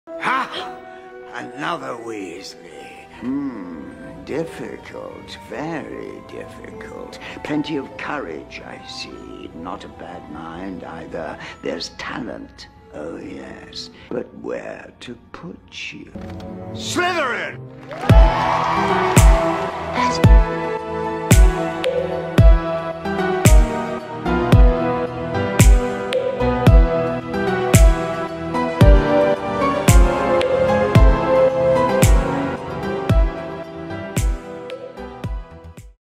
Ginny Weasly as a Slytherin?
Any Harry Potter fans out there? My friend wanted me to do this and so I decided to post it. I love all of the hogwarts houses for their distinct qualities. I’m a Gryffindor with Slytherin friends and I honestly believe they are misunderstood. But everyone is awesome, I hope y’all enjoy! 🙃😁 (not my photos.